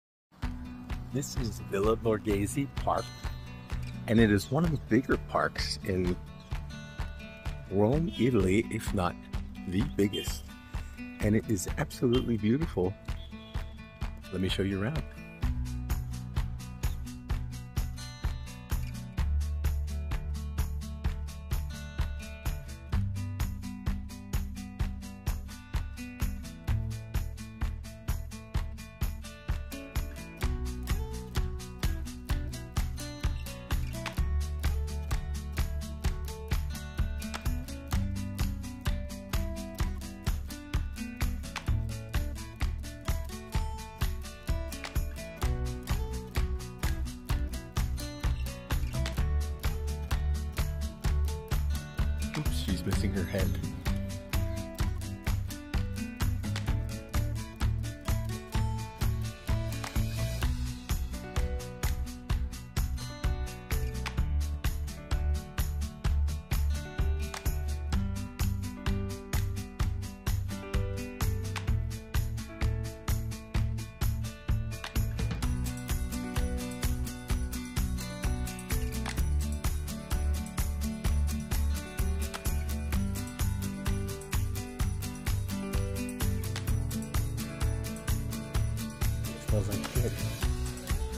Borghese Park, Rome, Italy!
Take a stroll with me through Borghese Park in Rome, Italy.